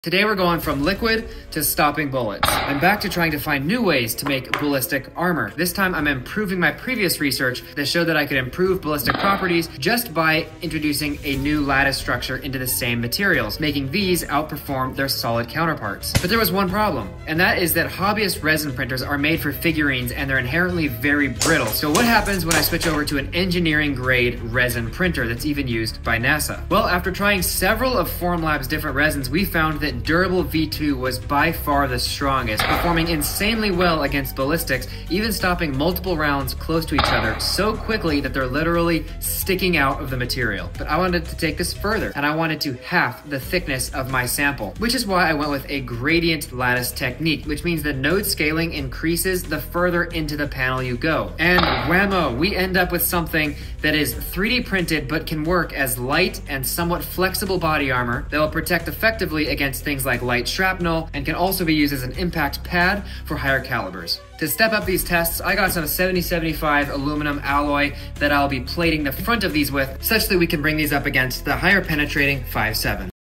BATMAN ARMOR💥🛠️
I’m pushing the limits of impact-resistant materials using super-tough resin from Formlabs + iterating some gradient based lattices for an insane mix of strength, lightweight design, and repeatability. This research could lead to complexly shapes armor panels which can have integrated strap mounts and even joints and other integrated mechanisms!  An immediate use case would be as next-gen armor backing layers. ✅

Full chaos on the main video—tons of ballistic tests & samples getting shot up! 💥